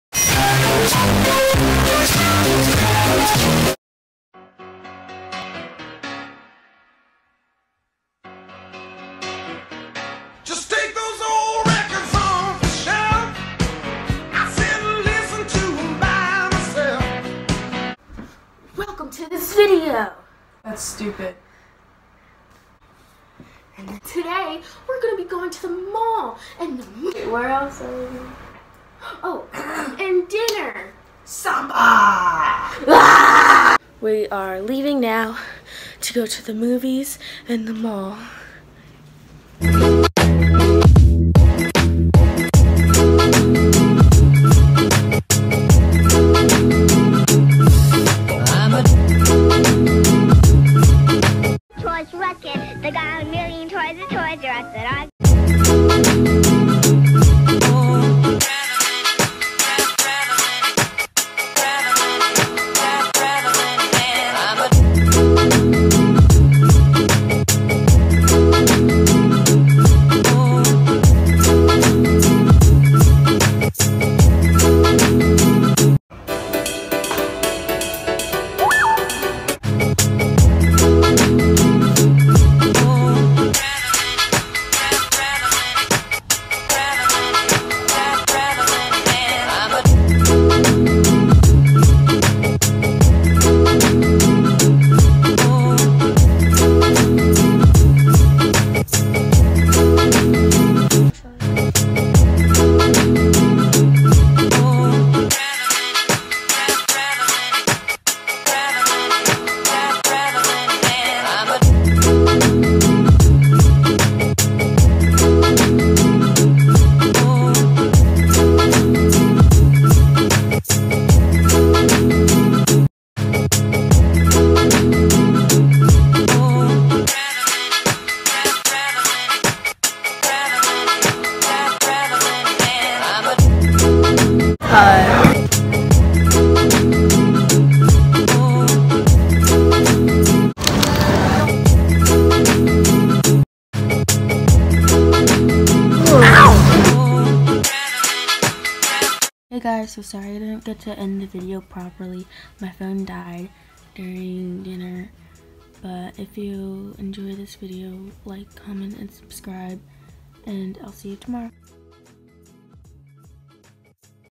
MY HAIR COUGHT ON FIRE AT SAMBA |Chloe Bridgeford
Today we went to see MaMa Mia 2, went to Old Navy, and out to dinner 🥘
I hope you guys enjoyed this second vlog